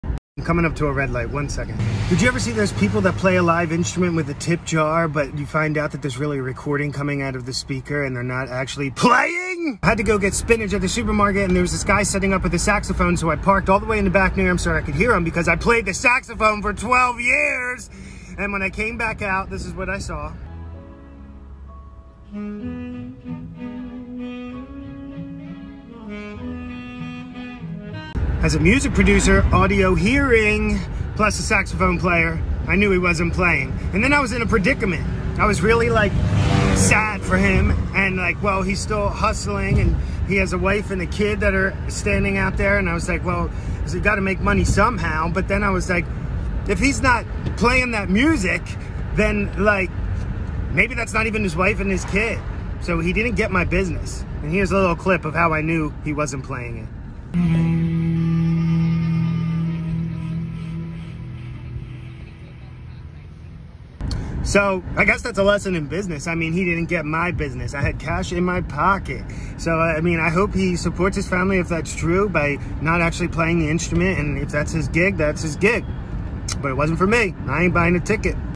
Guy Pretends To Play Sax For Tips 🤦‍♀️
This guy was pretending to play the sax for tips and I just couldn’t do it .